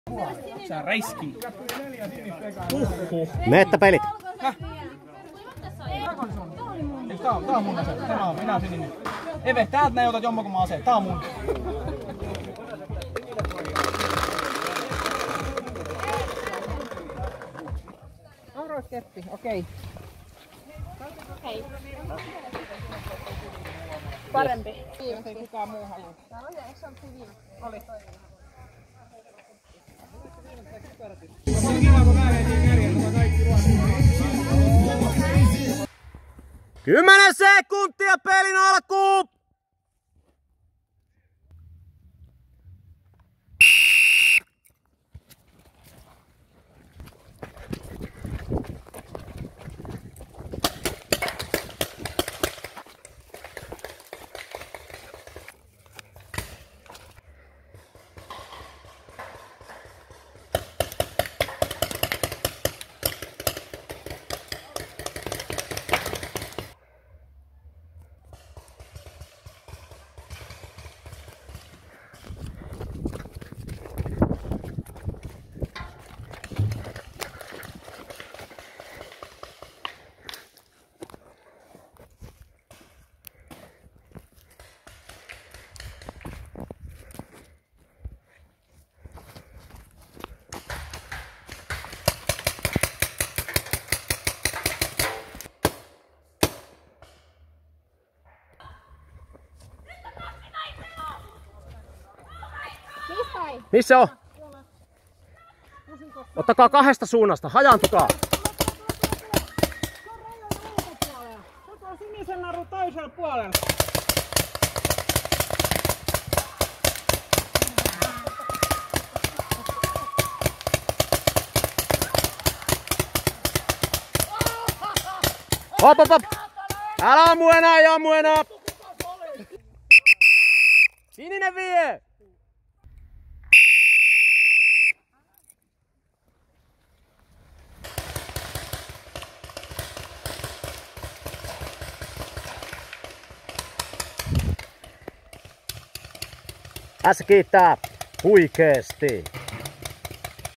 Innokkaat mimmit lahtasivat porukan kokeneimman kylmäksi.
Koskiko se tolppaa vai ei? Seikkailupuisto Huikeen porukka kävi värikuulahippasilla eikä armoa annettu - eikä pyydetty. Vauhdikkaat tykypelit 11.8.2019.

Suomen parhaat K7-, K13- ja K18-ohjelmat paintballissa ja kuplafutiksessa huippupuitteineen, -kalustoineen ja -vetäjineen tarjoaa Ässä Paintball & Kuplafutis ks.